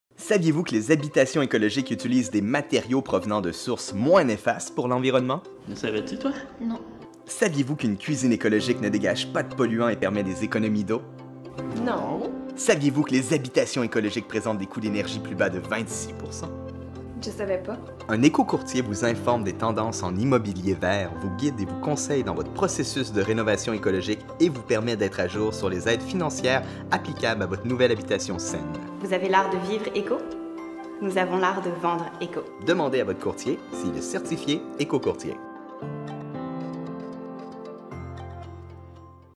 Via Capitale du Mont-Royal vous présente la certification ÉcoCourtier
Un écocourtier vous informe des tendances en immobilier vert, vous guide et vous conseille dans vos processus de rénovations écologiques et vous permet d’être à jour sur les aides financières applicables à votre nouvelle habitation saine.